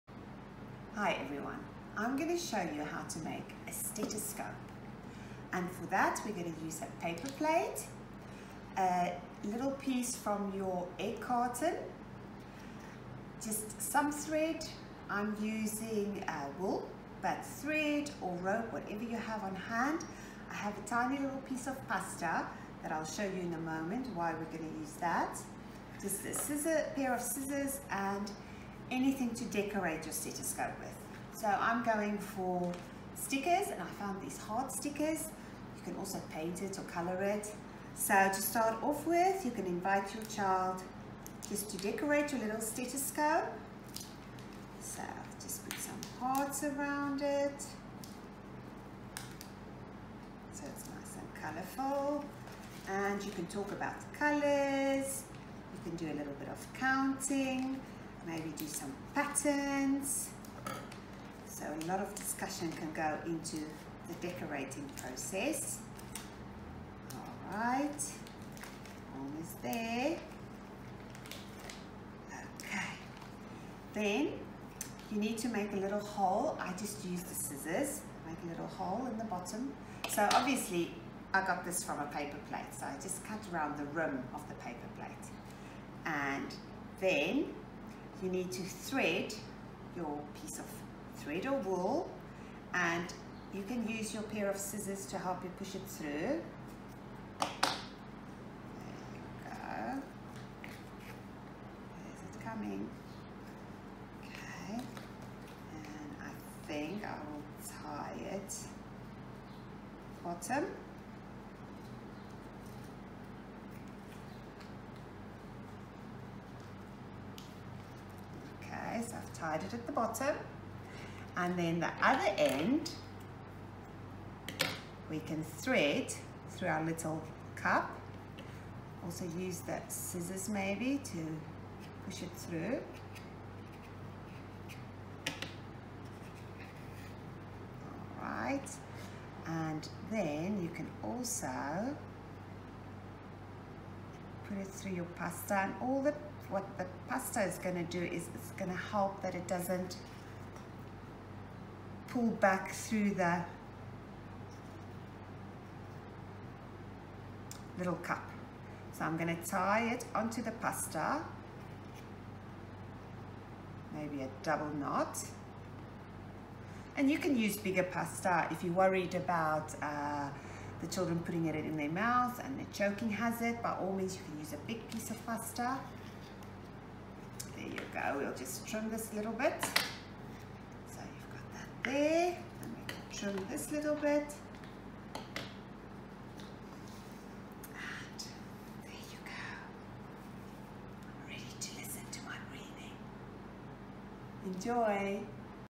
Activity - How to make a stethoscope
Join Hesta as she demonstrates a fun craft activity designed to give your little ones fingers a good workout and refine their hand eye coordination.